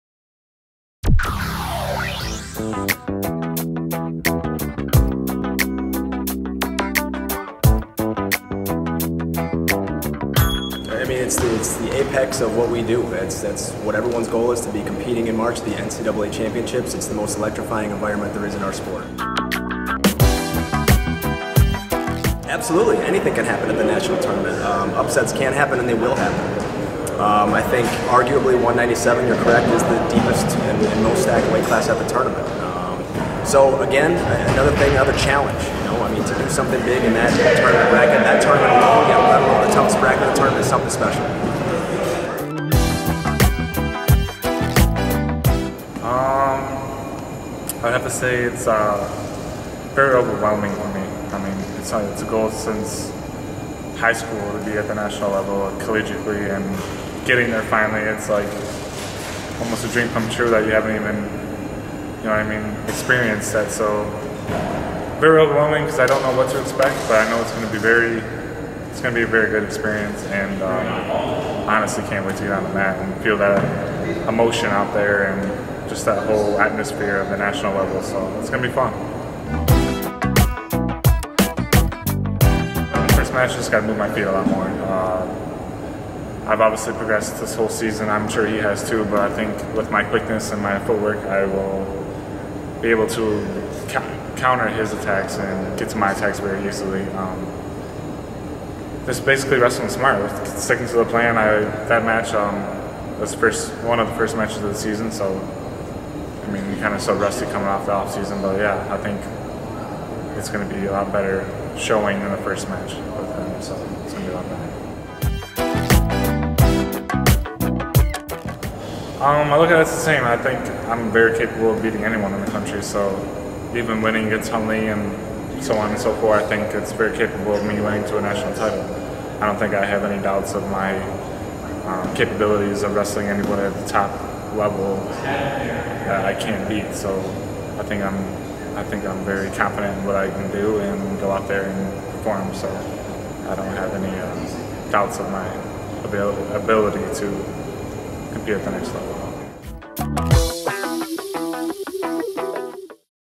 NIU Wrestling - NCAA Championships Preview
Ryan Ludwig and Shawn Scott discuss this weekend's NCAA Championships.